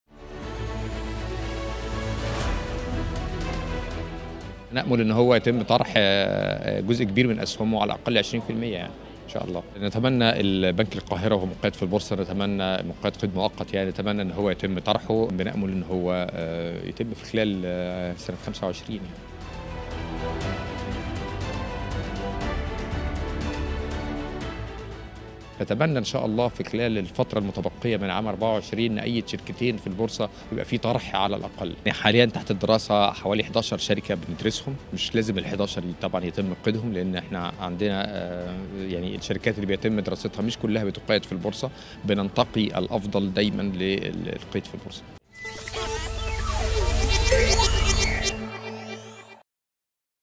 ماذا يتوقع رئيس البورصة المصرية لطرح المصرف المتحد وبنك القاهرة؟
توقع أحمد الشيخ رئيس البورصة المصرية، أن يتم طرح ما لا يقل عن 20% من المصرف المتحد، وأضاف:"نأمل أن يتم طرح بنك القاهرة أيضا خلال العام المقبل".

كما تخطط البورصة المصرية، بحسب الشيخ، لقيد شركتين، وطرح واحدة حتى نهاية العام الجاري، كما سيشهد عام 2025 دراسة 11 شركة لقيد المناسب منها في البورصة.

إيكونومي بلس